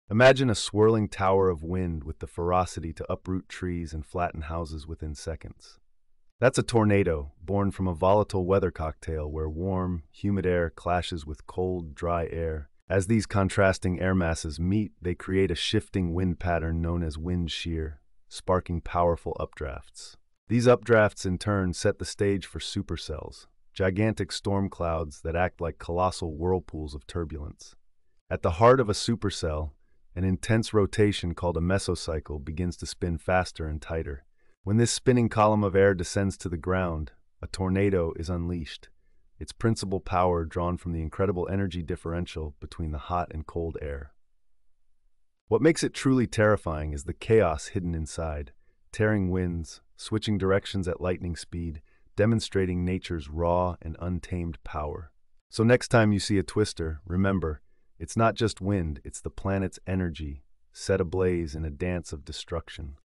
🌪 The Science of Tornadoes Unleashed!
Explore how tornadoes form and why they're so powerful. Discover the forces behind these natural phenomena and what makes them spin.